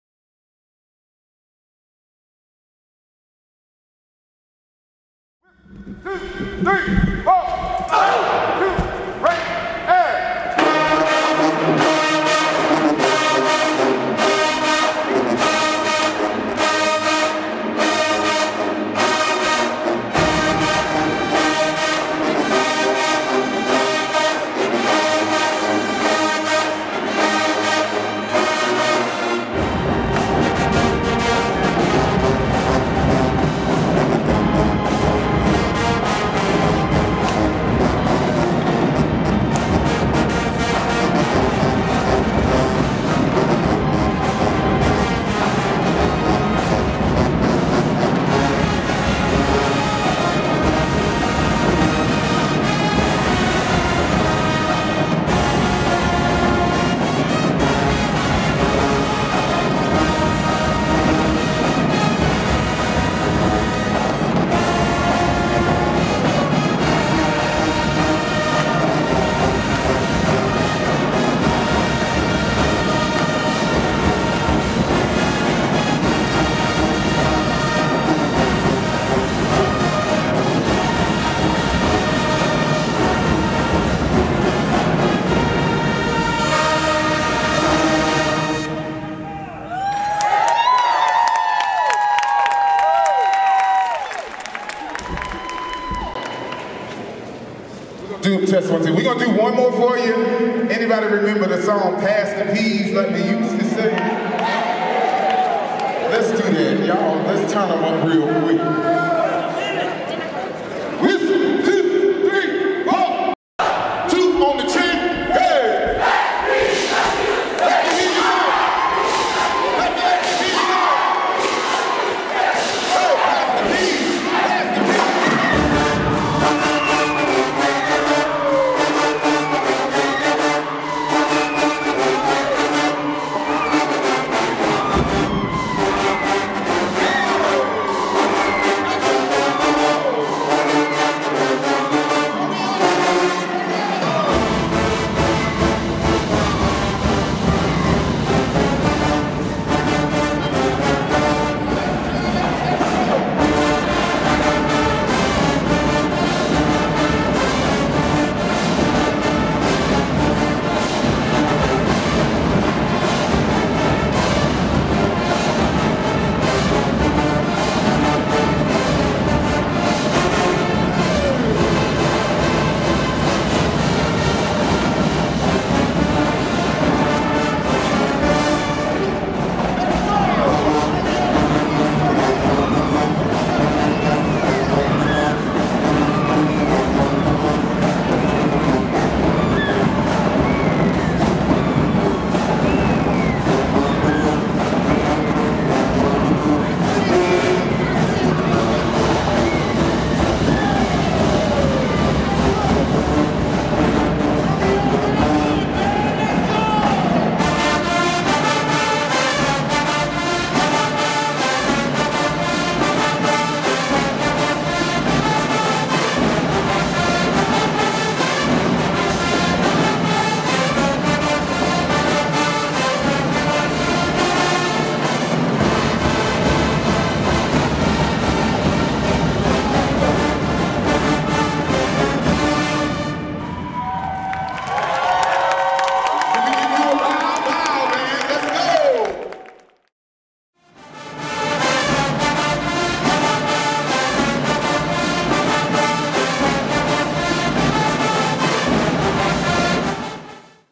Mass Band Performance | The Merge BOTB 2024
Footage from the Mass Band performance at The Merge Battle of the Bands hosted by the Kingstree High School River of Soul at the Florence Center in Florence, SC on April 13th, 2024.

Bands Featured:
Columbia HS Mighty Marching Eagles (Huntsville, AL)
Kingstree HS River of Soul (Kingstree, SC)
Woodland HS Marching Wolverines (Dorchester, SC)
Cross HS Marching Trojans (Cross, SC)
W.J. Keenan HS Marching Raiders "Rubber" Band (Columbia, SC)
Cross Keys HS Tribe of Funk Marching Band (Atlanta, GA)
Timberland HS Marching Wolves (St. Stephens, SC)
Mallard Creek HS Marching Eagles (Charlotte, NC)

finesseproductions.shop/
$PenderEnterprises